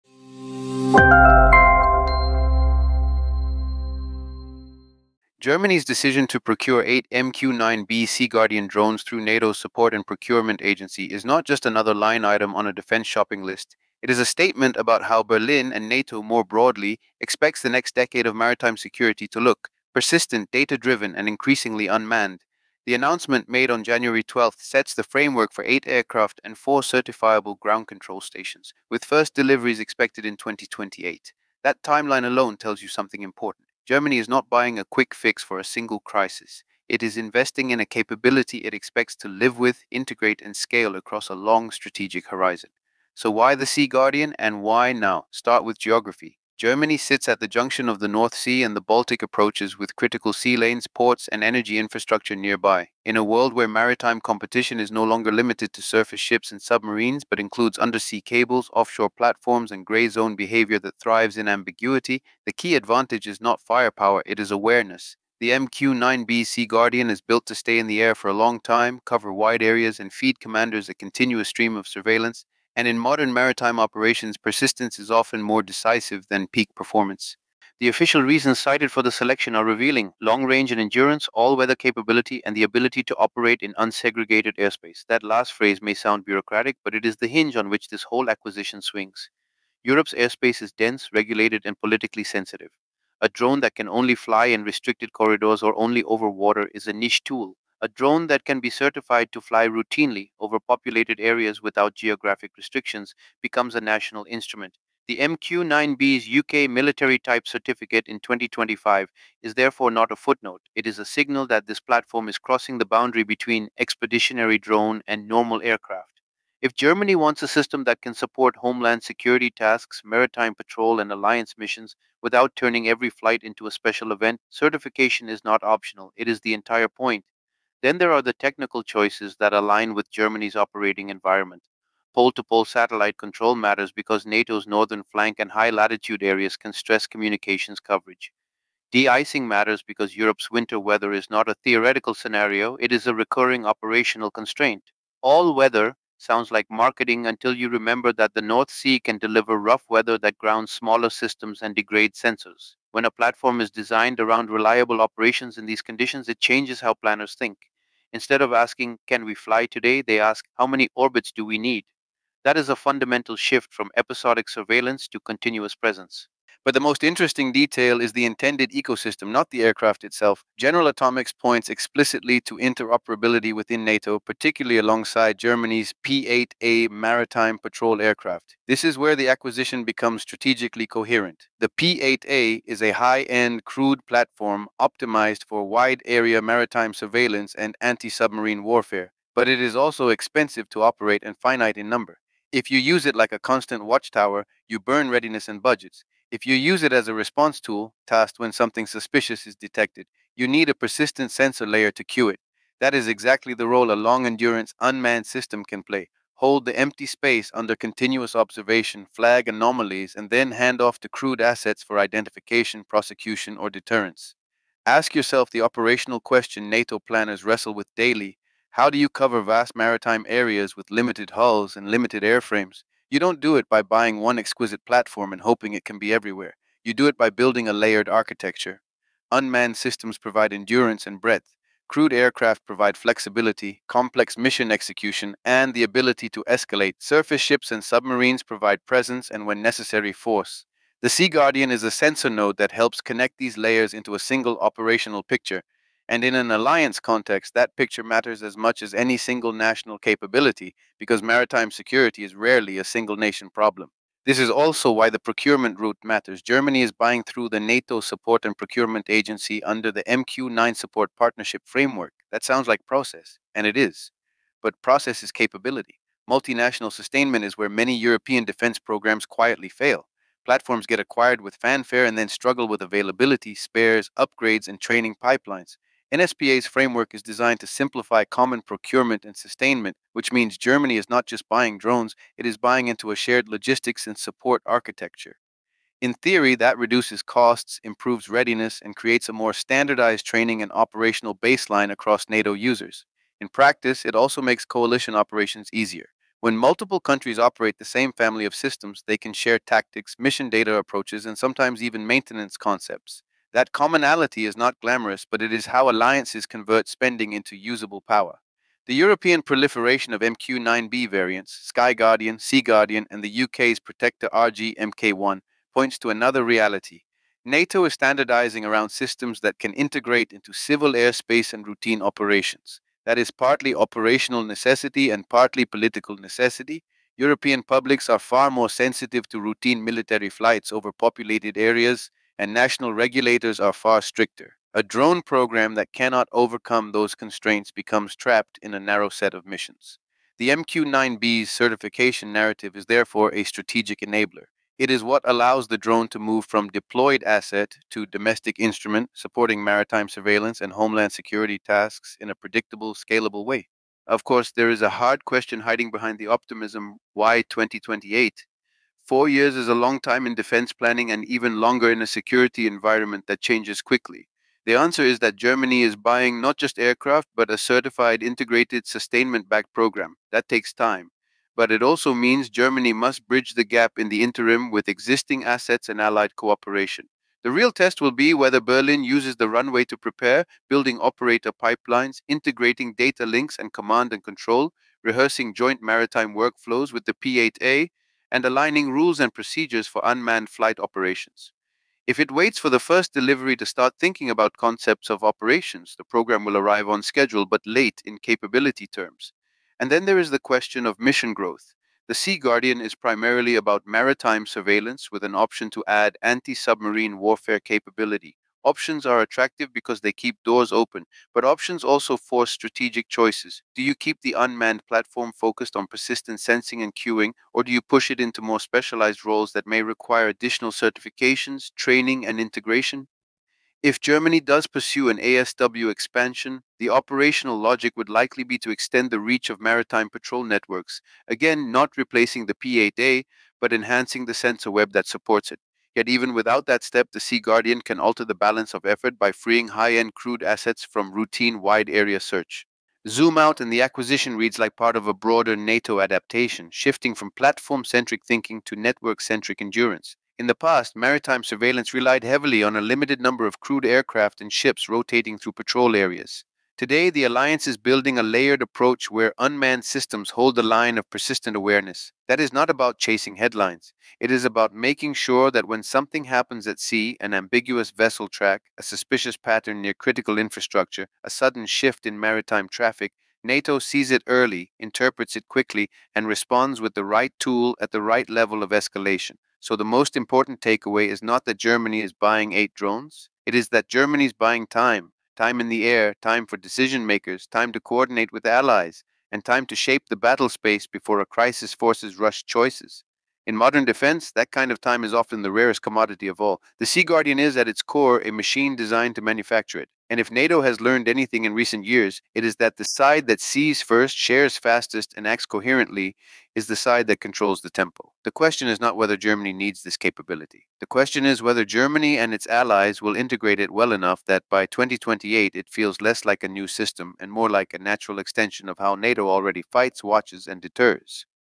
Why Germany’s MQ 9B SeaGuardian Deal Matters for NATO Maritime Power
Germany has agreed to acquire eight MQ-9B SeaGuardian drones via the NATO Support and Procurement Agency (NSPA), with first deliveries expected in 2028. On paper, it looks like another UAV purchase. In reality, it signals a major shift in how NATO plans to monitor the North Sea, Baltic approaches, and critical maritime infrastructure—using persistent, all-weather surveillance and tighter interoperability across allied fleets.

In this analysis, we break down why the MQ-9B SeaGuardian was selected, what “unsegregated airspace” operations and certification really mean for Europe, and how this system complements Germany’s P-8A maritime patrol aircraft. We also examine the NSPA MQ-9 Support Partnership framework—why sustainment and commonality can be more decisive than raw performance—and what the optional anti-submarine warfare (ASW) pathway could imply for future NATO maritime architecture.

If you want to understand how modern deterrence increasingly depends on endurance, sensors, and shared data pipelines—not just ships and missiles—this is the story to watch.